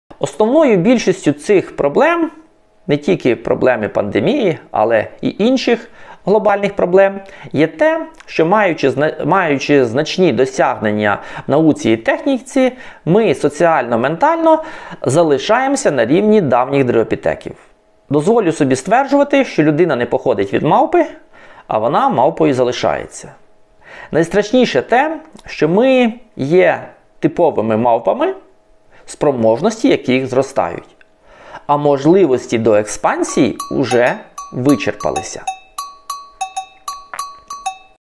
Іван Хом'як про головну проблему людства
Іван Хом'як. Кандидат біологічних наук, доцент кафедри екології та географії Природничого факультету Житомирського Державного університету імені Івана Франка.